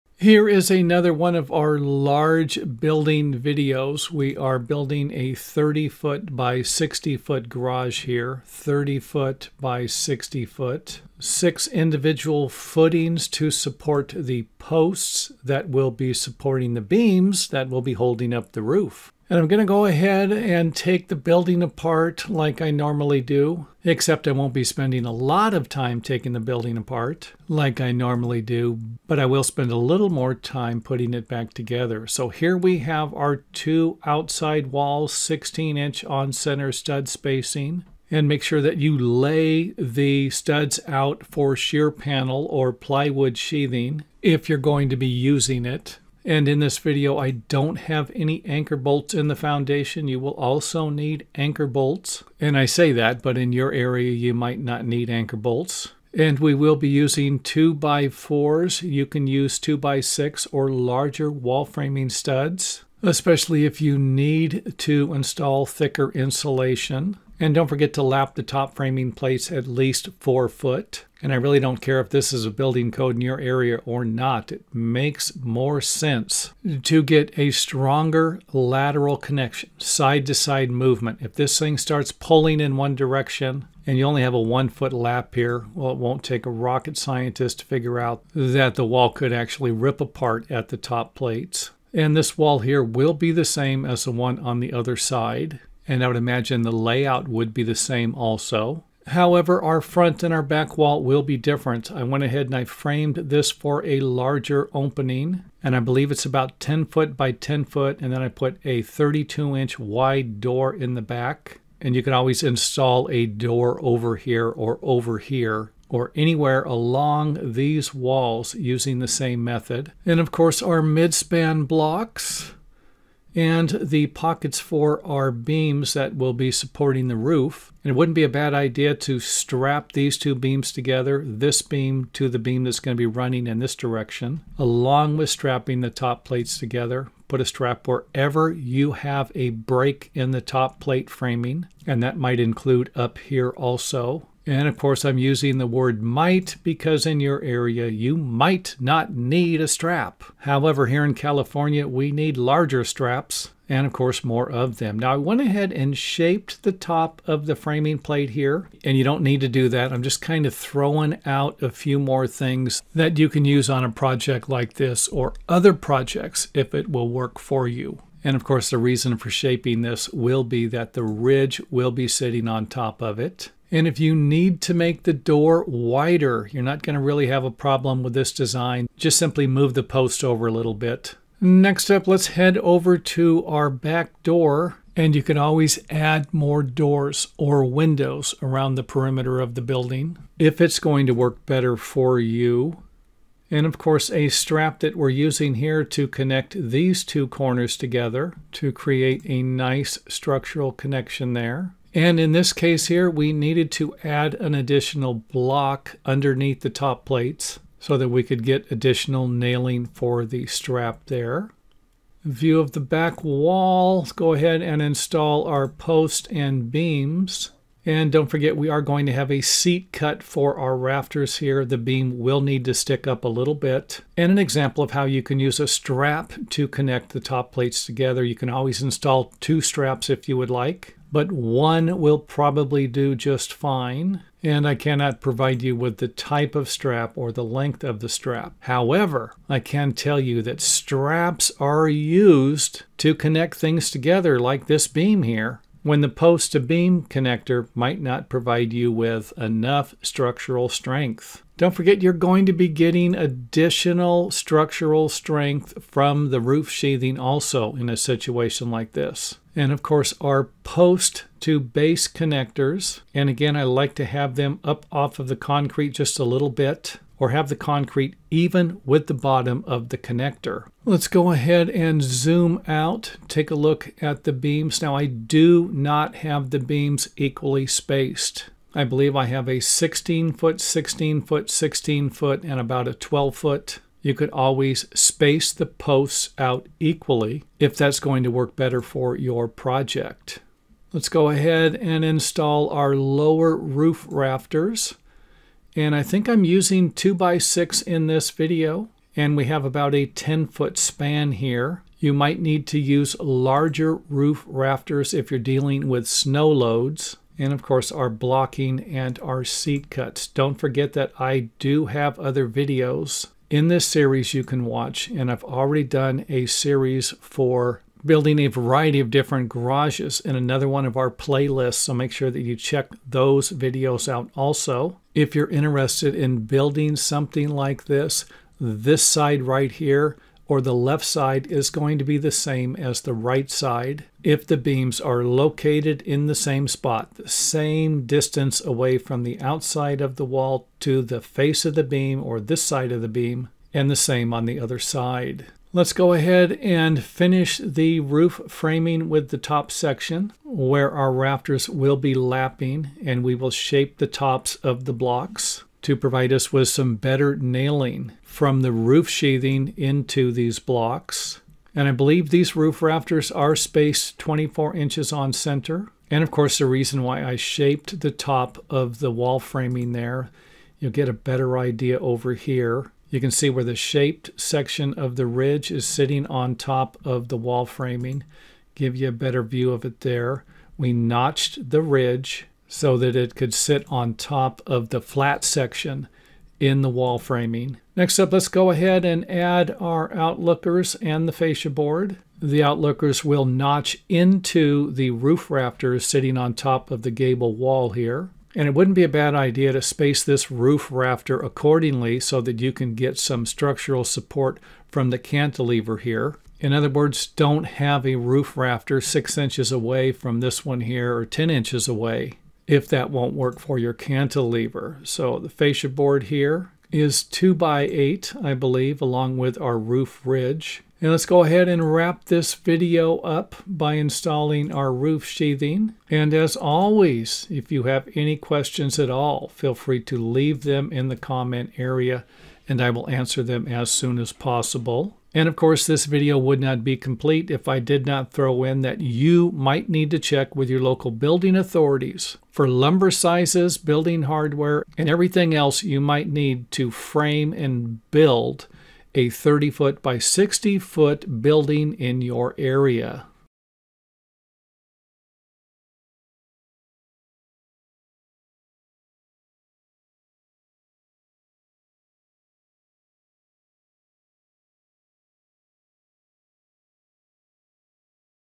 How To Frame And Build 30 x 60 Foot Garage, Workshop And Storage - Construction Education Examples
Here's an example of how a 30 x60 foot garage or workshop can be built without roof trusses. We are using beams and roof rafters instead. Make sure you check with local building authorities and engineers before building anything like this in your area.